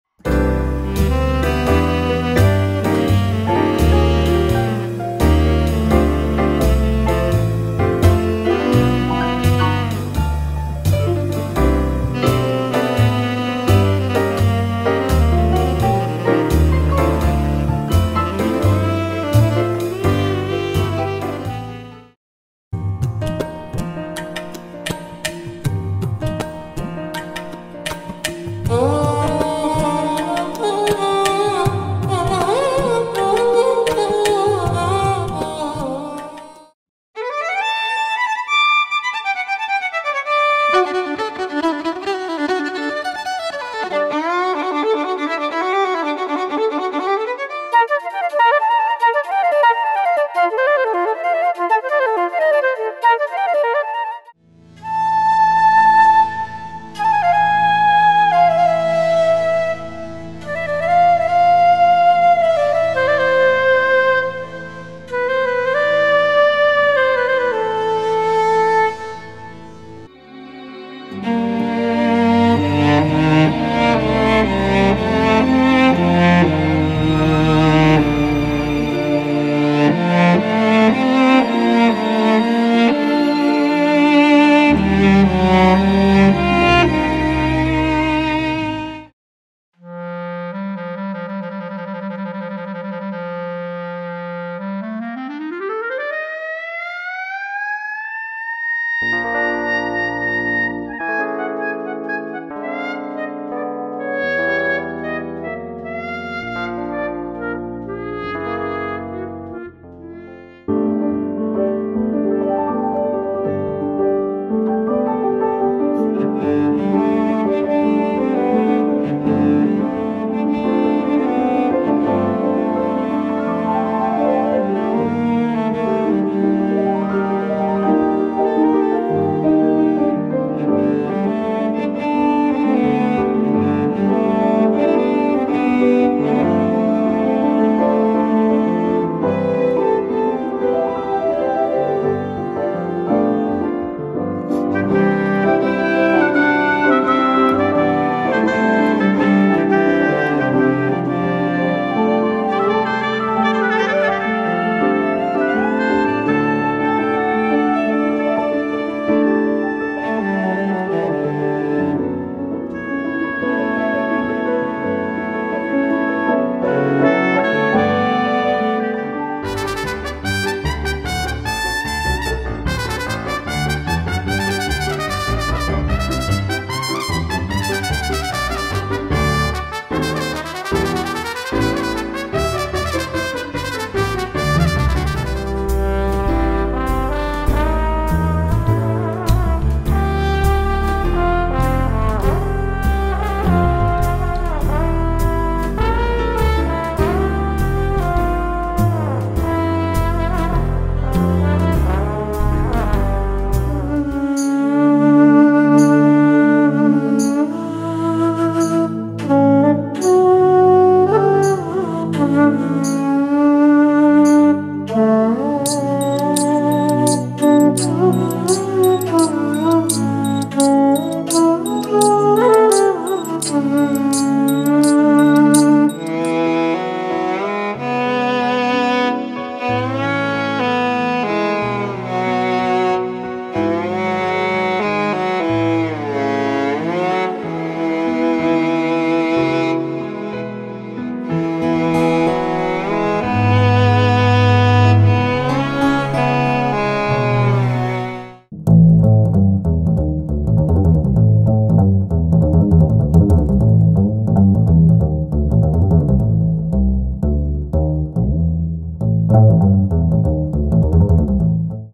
GeoSWAM Collection I and II Preview
GeoShred 5 with 11 beautifully inspired physically modeled instruments, including GeoViolin, GeoCello, GeoViola, GeoPizzBass, GeoFlute, GeoBassFlute, GeoTenorSax, GeoClarinet, GeoOboe, GeoTrumpet and GeoBassTrombone.  All powered by Audio Modeling's SWAM technology. These instruments are available individually as In-App-Purchases (IAPs), for $14.99USD**

The GeoSWAM instruments are awesome to play from GeoShred's isomorphic keyboard, and you can also play them from other expressive MPE controllers like the LinnStrument or the Roli Seaboard.

**Note: All Prices are in US Dollars (USD). Apple sets the local territory price and may add additional local charges such as VAT, GST, HST.  Check the store in your territory for the local price.